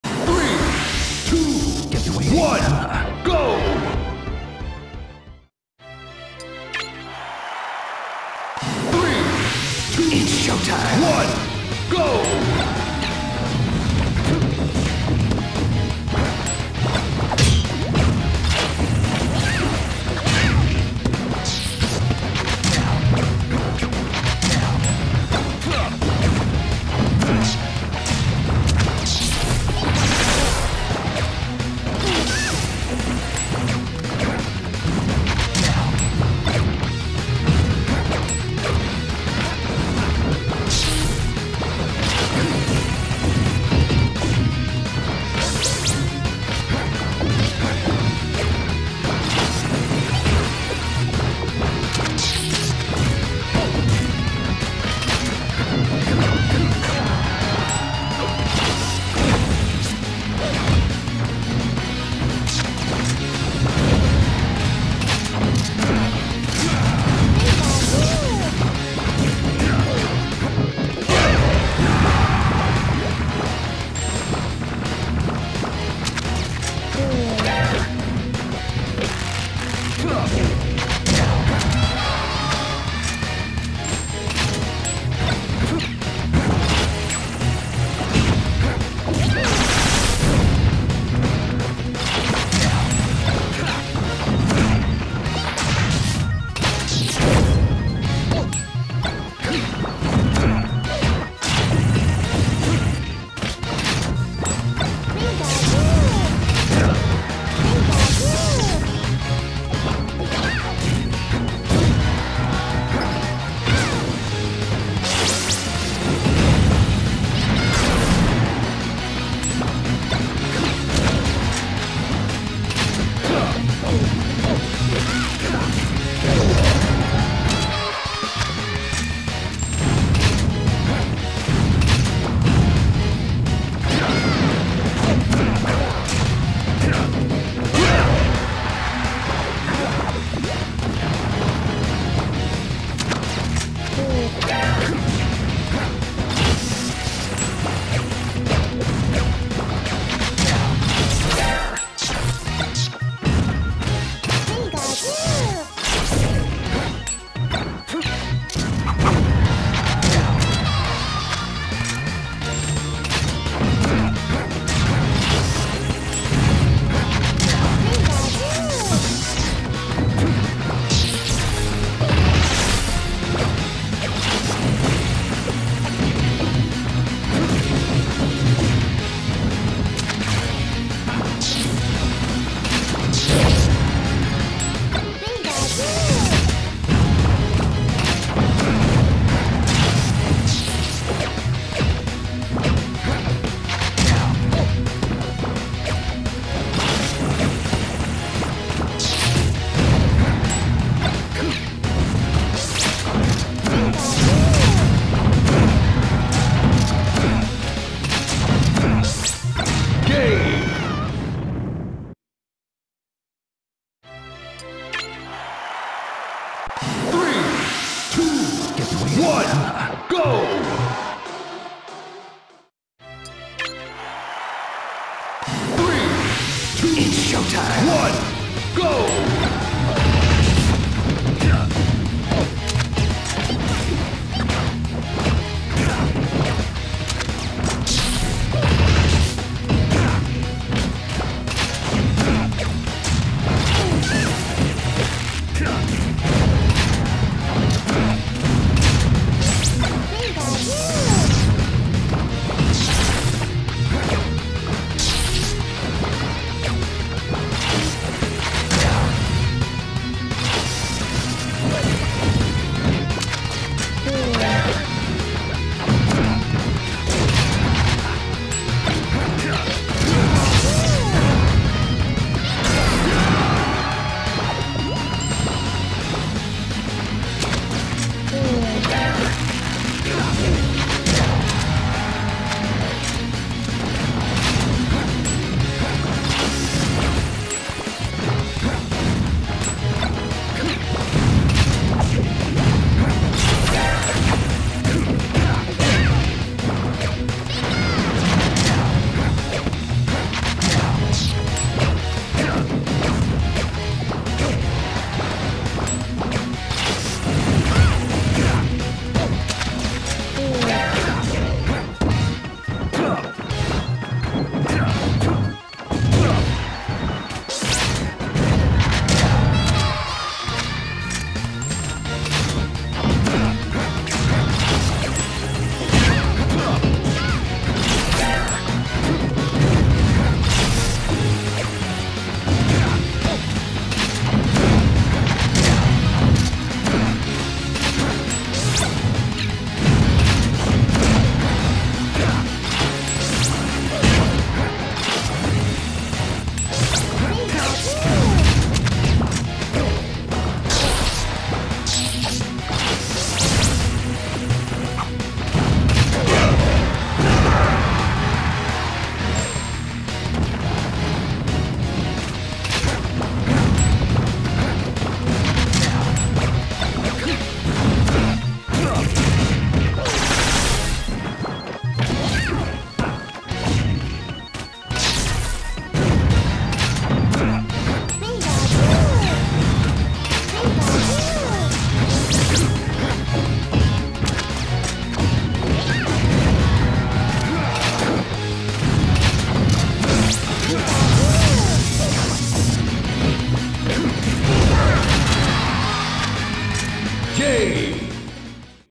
ilikepizza107 (Snake) vs Niko (Pikachu) - Southside Sundays 44 - Losers Round 3
Southside Sundays 44 (6/11/23)